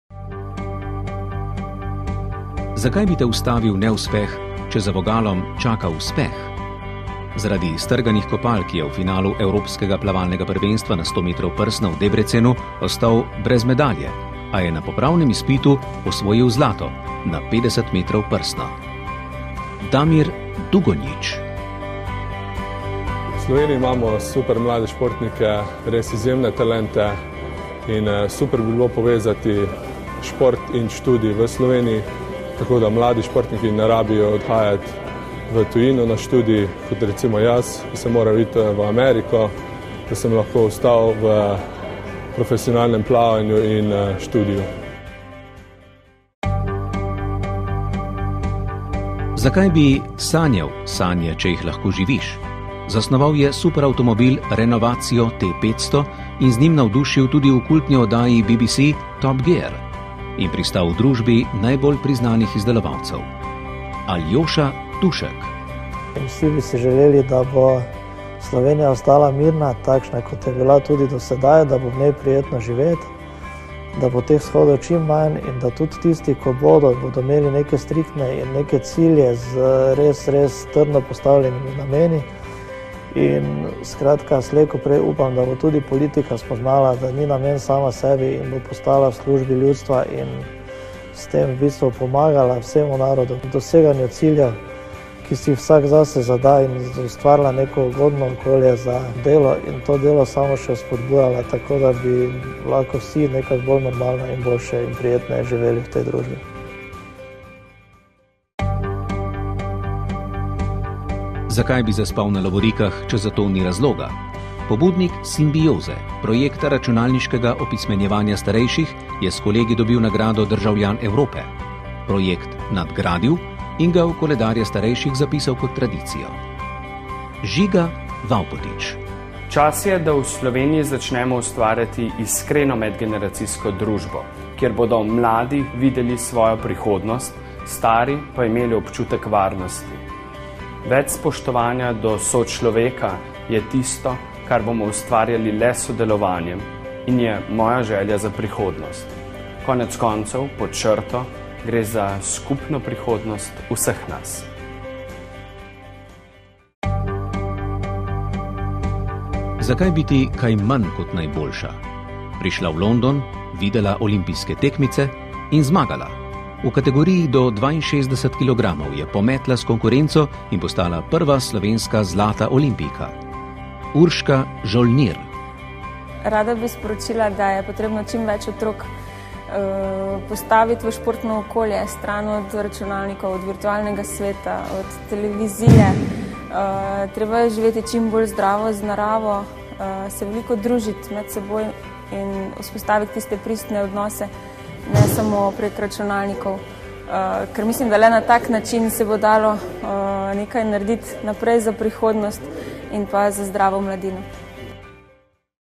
Val 202: Finalisti za Ime leta 2012
Akcija Vala 202 - Ime tedna / Ime meseca / Ime leta

PORTRETI
Ime maja 2012: Damir Dugonjić
Ime junija 2012: Aljoša Tušek
Ime julija 2012: Žiga Vavpotič
Ime avgusta 2012: Urška Žolnir